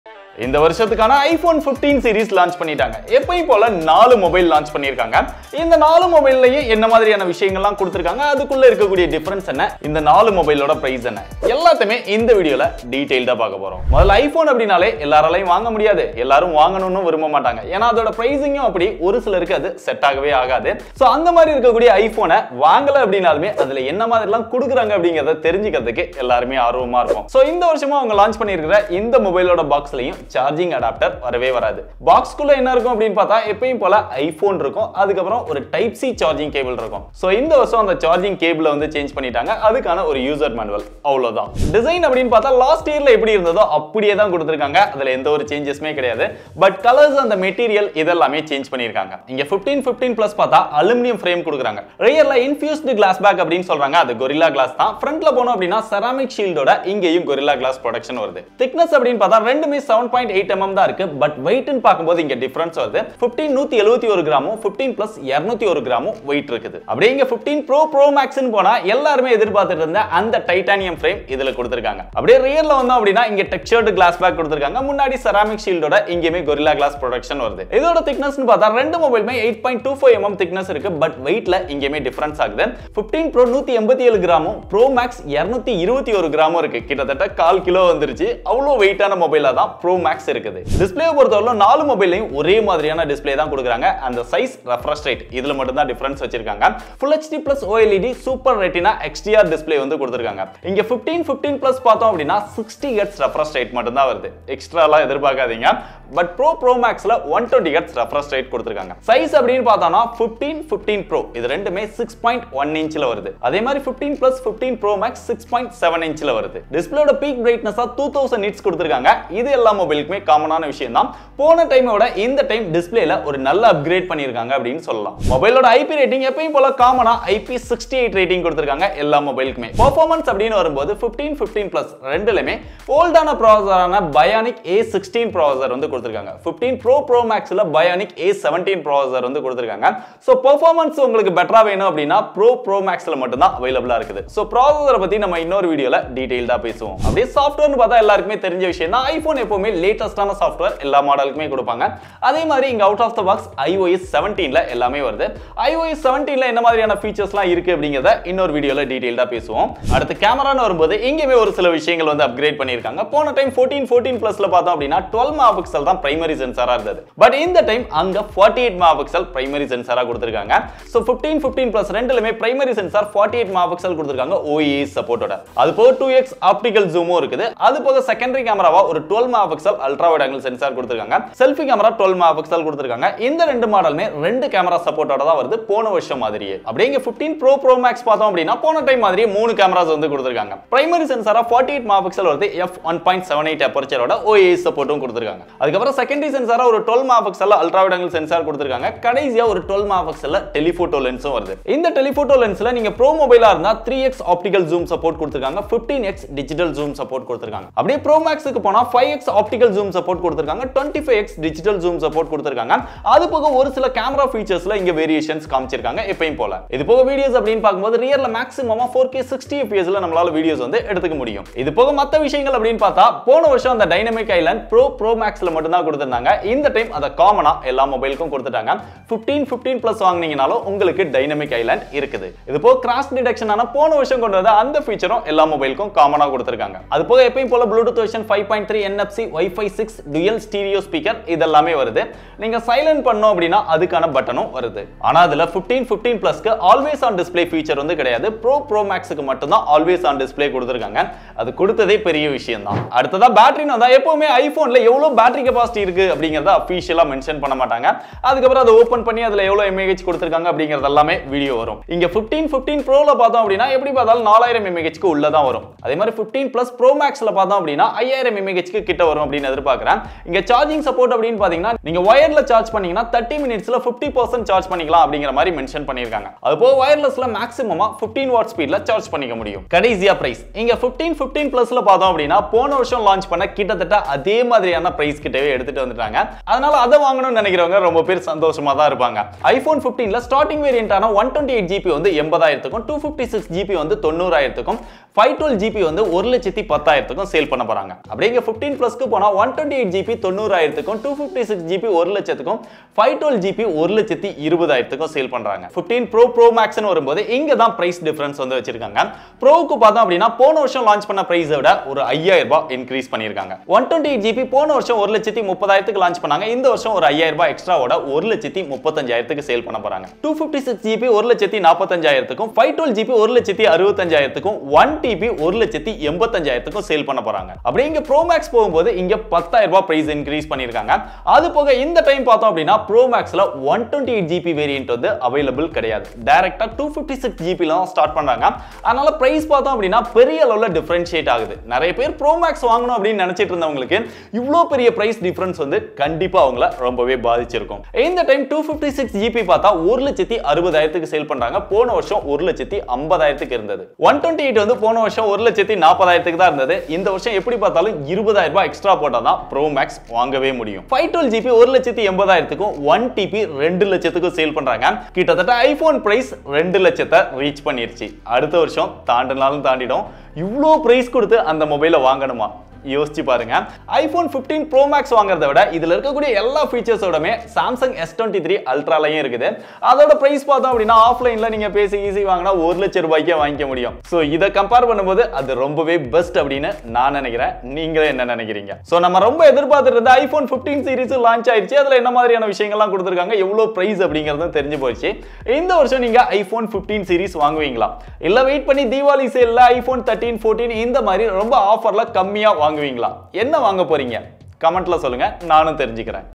iPhone 15 Series - அட்டகாசமா இருக்கு! ஆனா?😞 iPhone 15 Series Specs, Price & Comparision 😎TB
iPhone 15 Series - அட்டகாசமா இருக்கு! ஆனா?😞 iPhone 15 Series Specs, Price & Comparision 😎iPhone 15 vs iPhone 15 Plus vs iPhone 15 Pro vs iPhone 15 Pro Max🤩Explained